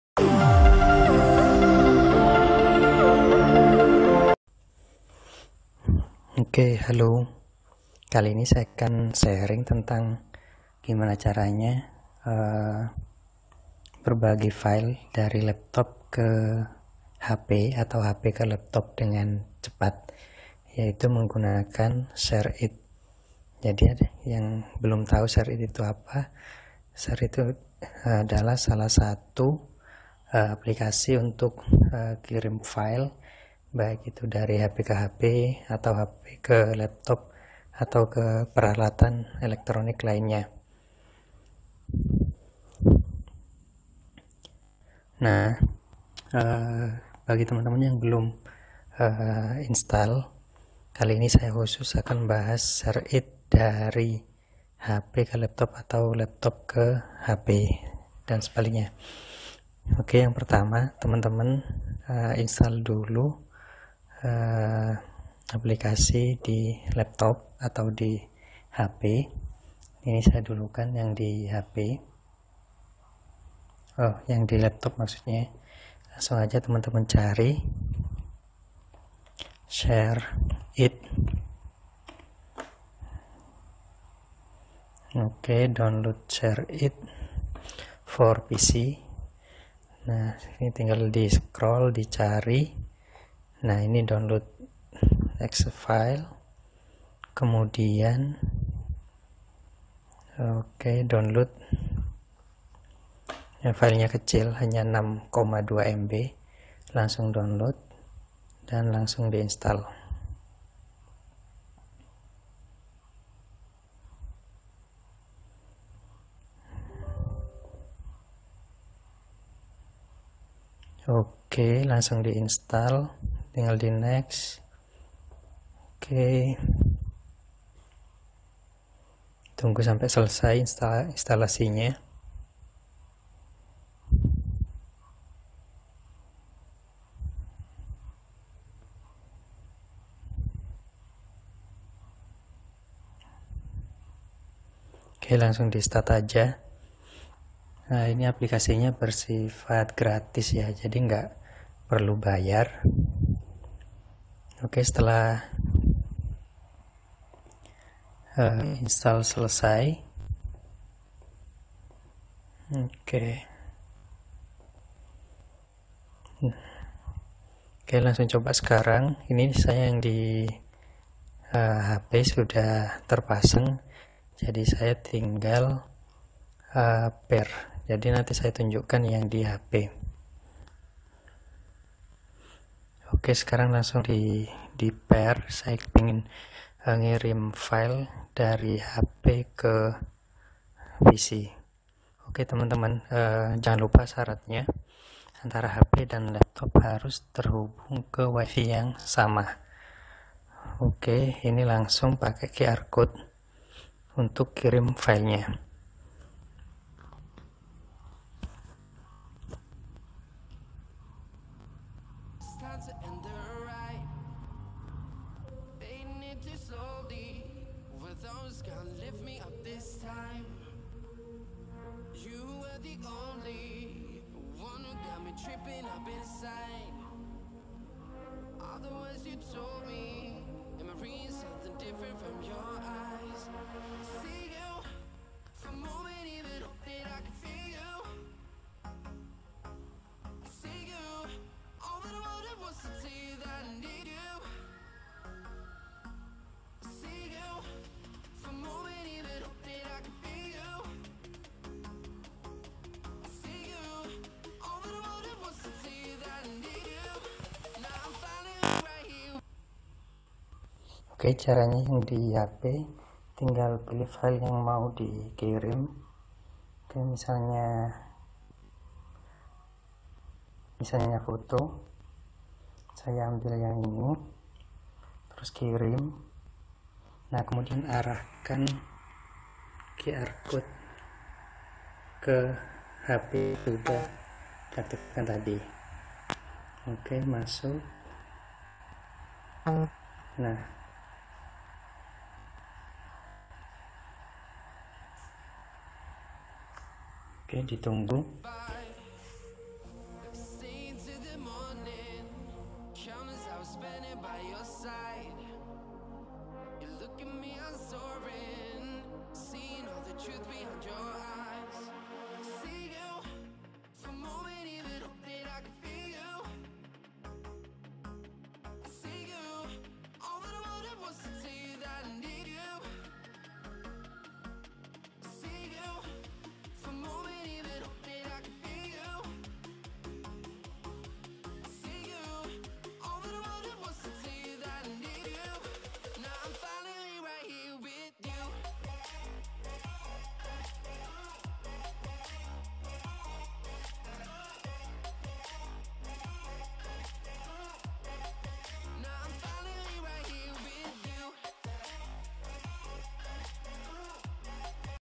Cara Kirim File dari HP ke Laptop Tanpa Kabel dengan Cepat
Cara Kirim File dari HP ke Laptop Tanpa Kabel dengan Cepat Menggunakan Share it, sobat hanya butuh jaringan wifi yang sama antara laptop dan hp.

How to Send Files from Cellphones to Laptops Without Wires Quickly Using Share it, you only need the same wifi network between your laptop and cellphone.